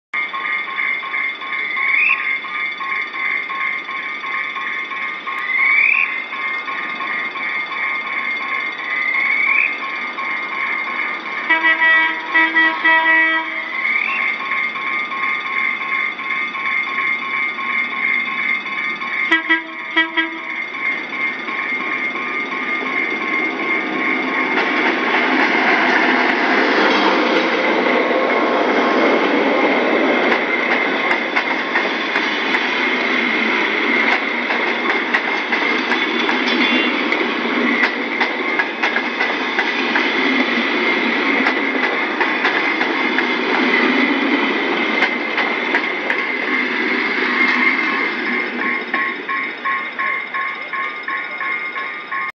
El Tren Nº111/115 con GT22 9082 de Ferrobaires por Merlo
El Tren Nº111/115 con destino Bragado y estaciones intermedias a cargo de la locomotora EMD GT22 9082 pasando por Merlo con la siguiente formacion: FCR1811+P1127+CT1404+CR1417+CT1353